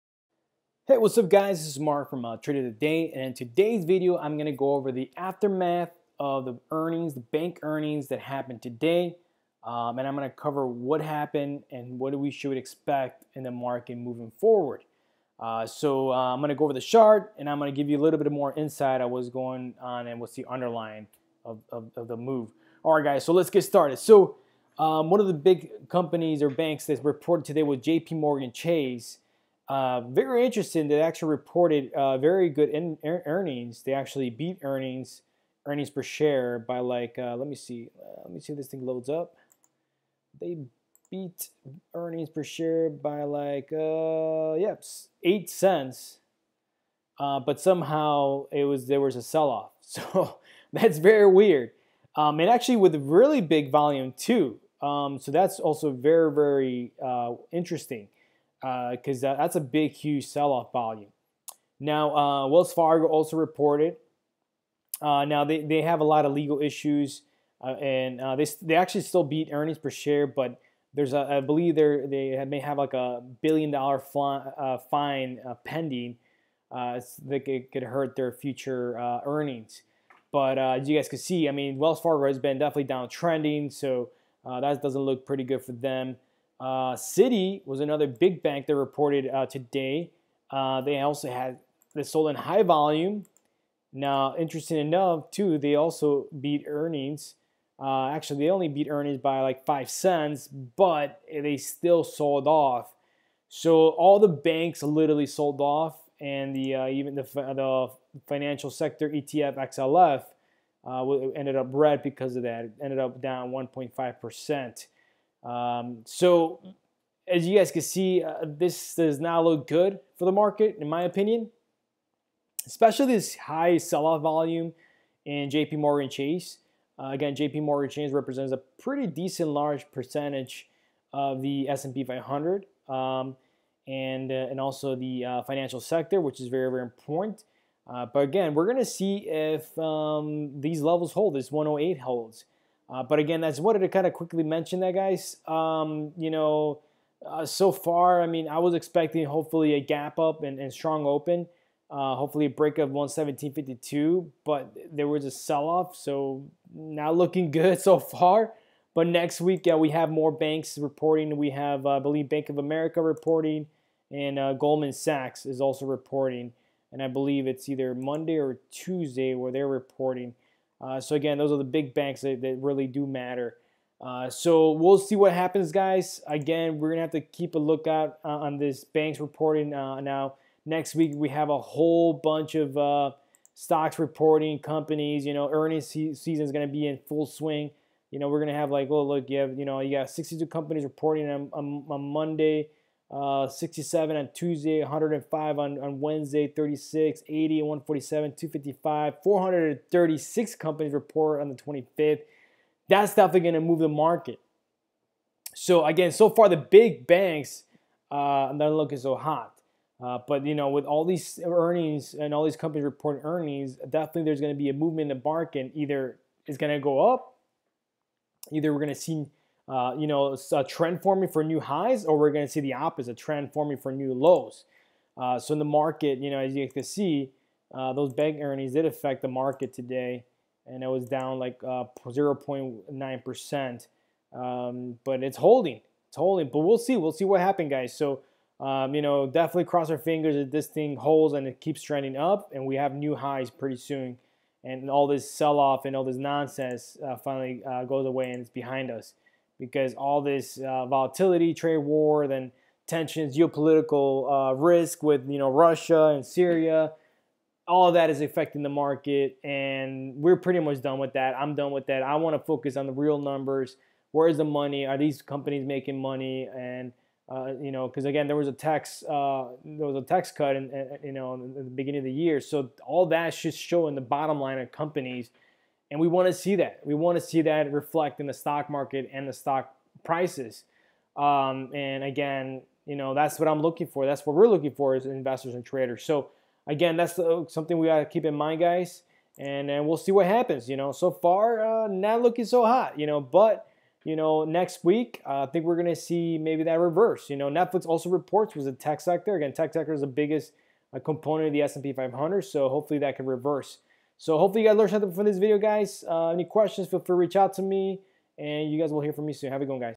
Why the Big Banks Earnings Report not helping the Stock Market?
In this video I will cover why the Big Banks Earnings Report not helping the Stock Market. I am going to cover the move the on the Financial Sector and what to look for next week in terms of Earnings.